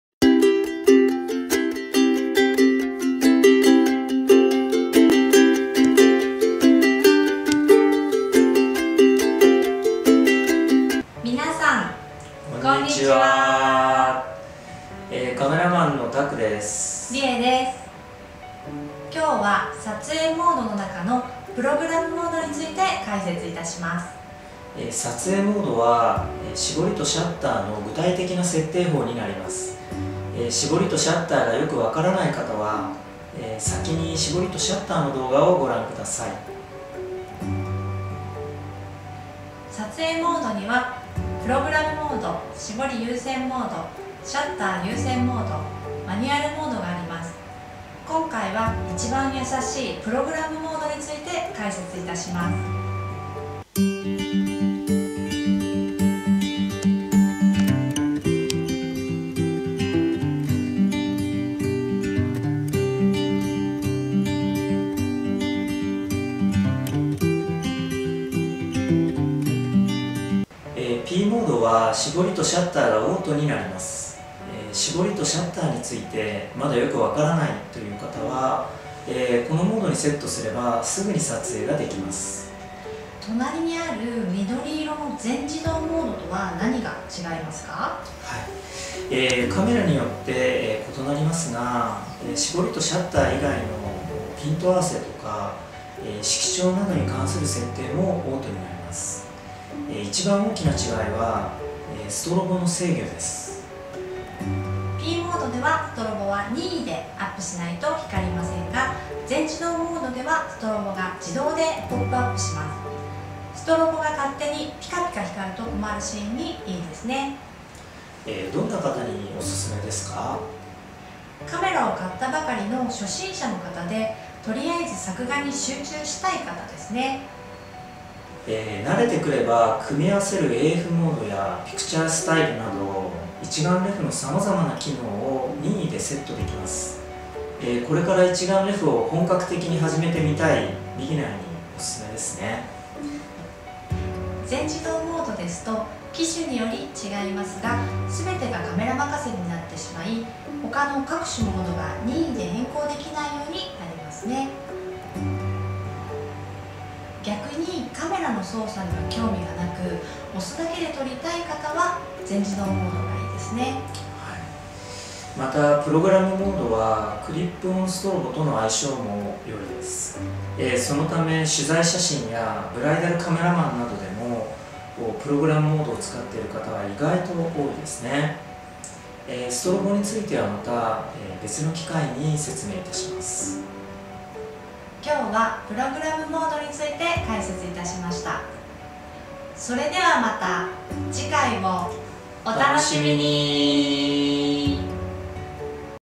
【ビギナーフォトレッスン】No.009／Pモード(プログラムモード)
カメラマン、タクとリエのフォトレッスンです☆
初心者向けのフォトレッスン第９回目。
第９回目は「Pモード(プログラムモード)」です。

☆初心者向けなので、動画をレッスン１から見れば誰でも写真撮影が上達します！！

数十年、写真教室（主に女性向け）を開講し、プロカメラマンをたくさん輩出した、講師：タクが、普段教室で教えているとってもわかりやすい内容を動画にしました！
リエは、子ども専門のカメラマンで、子ども写真の撮り方をお教え予定です。（←たいぶ先になります 笑）

※撮影するときは、サングラスは外しましょう^^
※画質が悪い場合は、視聴時に【設定】から【高画質】をお選びください。